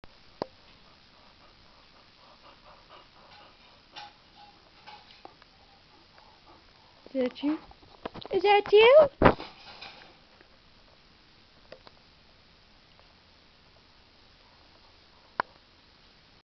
Love from BB the Boxer dog
Beriah gives you some love!! :) From one boxer love to another!!